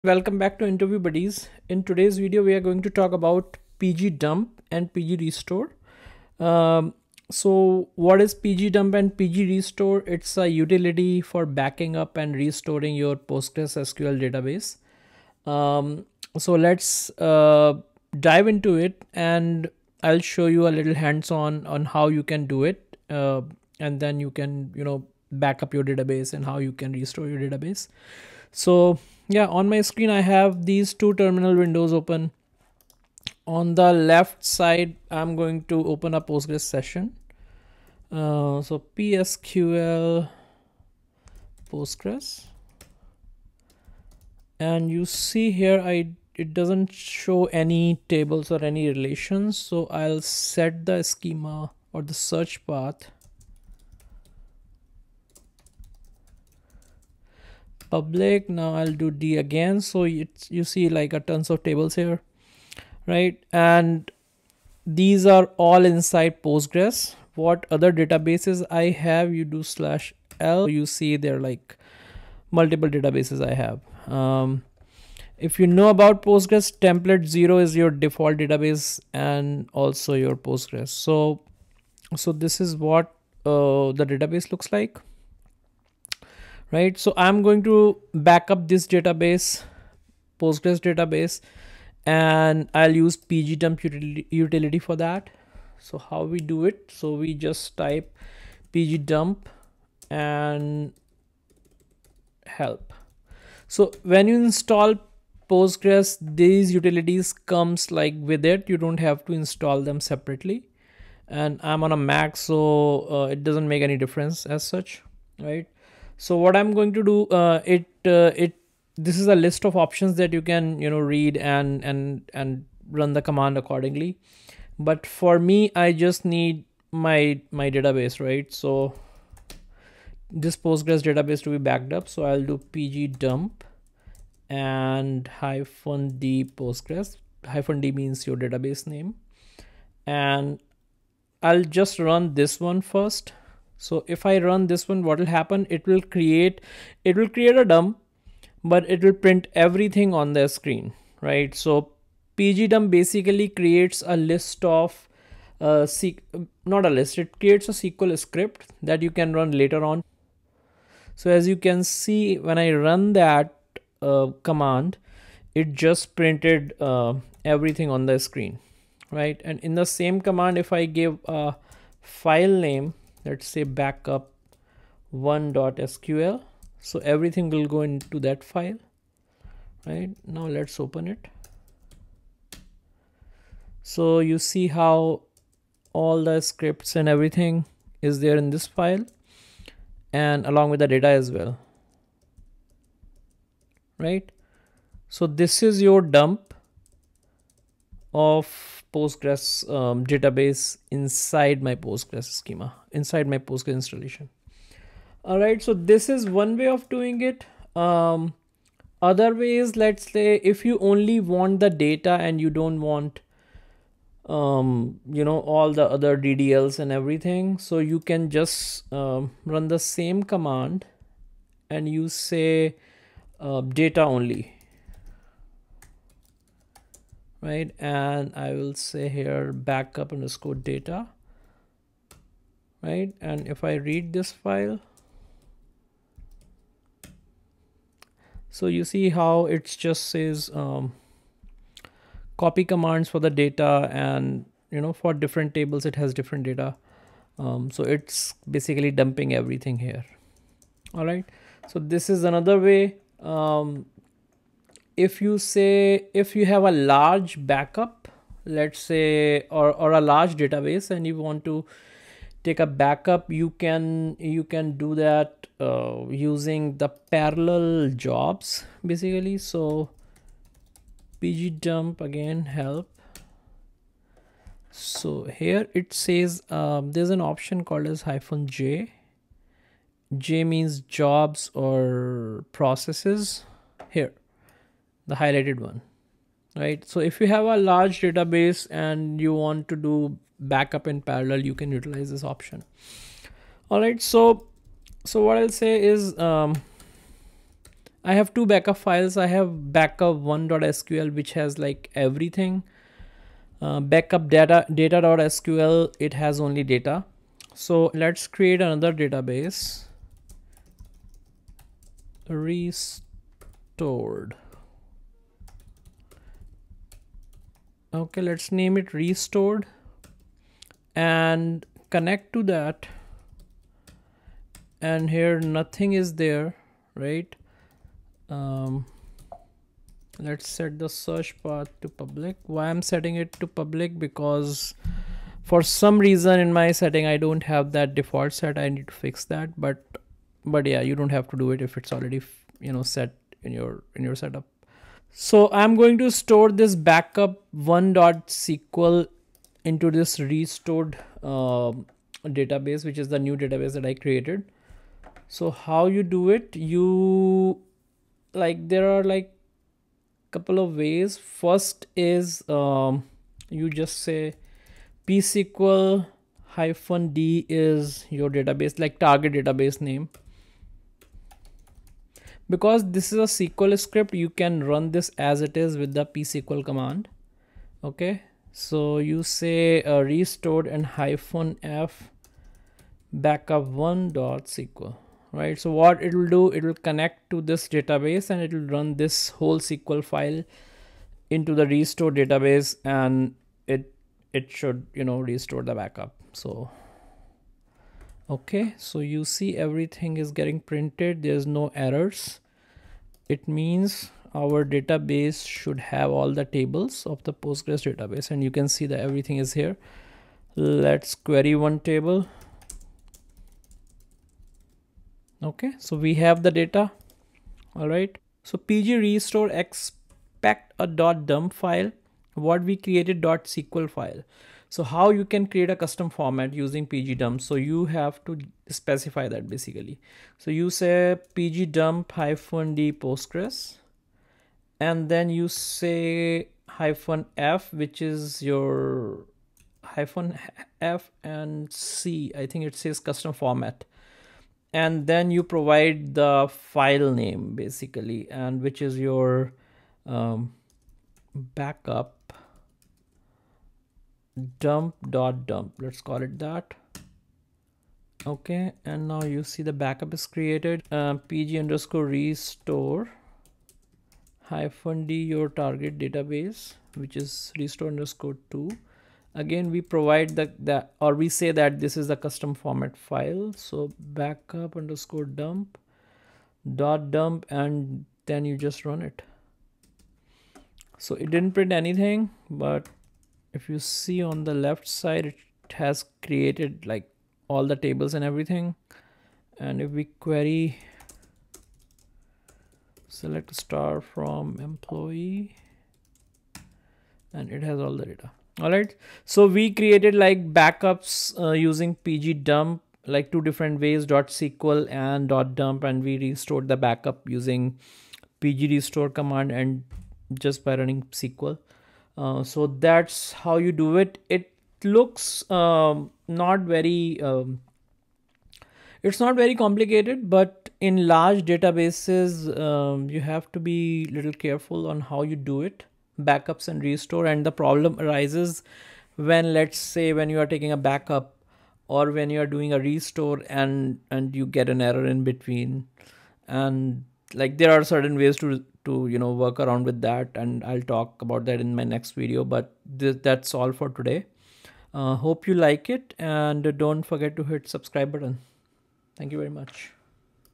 Backup and Restore PostgreSQL Database: pg_dump and pg_restore Explained
Learn how to efficiently back up and restore PostgreSQL databases using pg_dump and pg_restore. This tutorial covers the difference between SQL and custom dump formats, how to create .dump files, and when to use psql vs pg_restore for restoration. Watch to master essential PostgreSQL database management techniques and avoid common pitfalls during data backups.